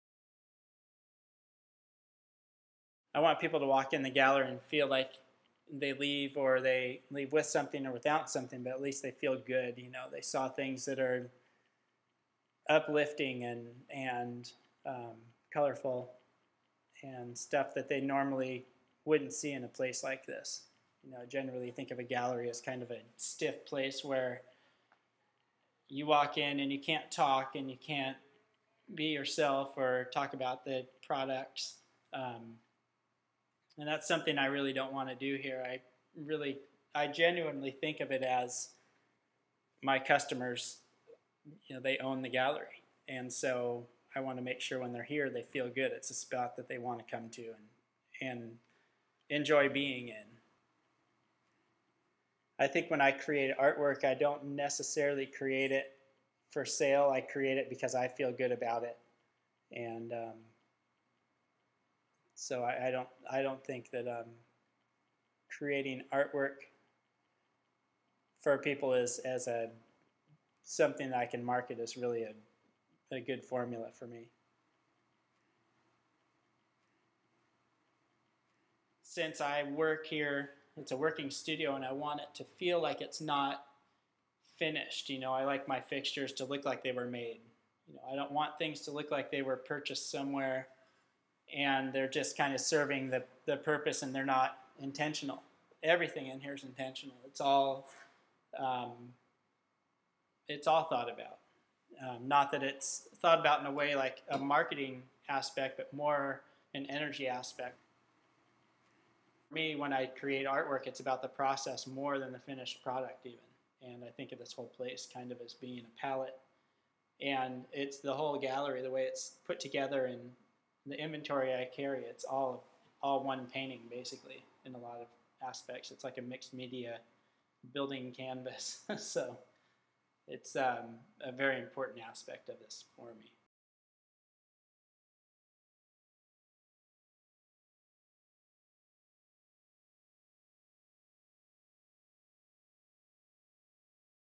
Fiona Bleu Gallery in Morro Bay California
Fiona Bleu is a vibrant, alive gallery in Morro Bay that features the art work of Rowan Chase and other like-minded arts.  The Gallery also features a modern furniture, artisan jewelry and other modern gifts.  Don't visit Morro Bay without seeing Rowan and Fiona Bleu.